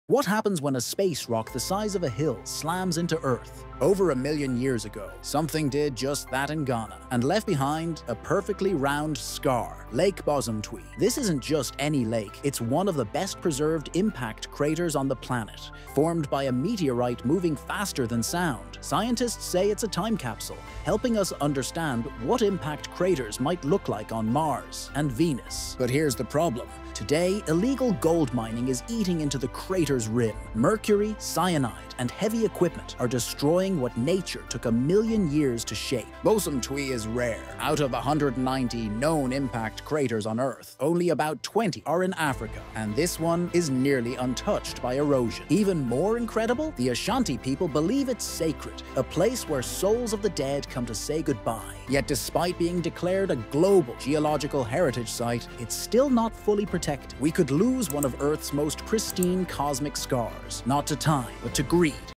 Earth’s Forgotten Crater: A Cosmic Scar in Danger
A million years ago, a meteor slammed into Ghana — creating one of Earth’s best-preserved impact craters. But Lake Bosumtwi is now under threat from illegal gold mining. Here's why scientists—and space agencies—want it protected.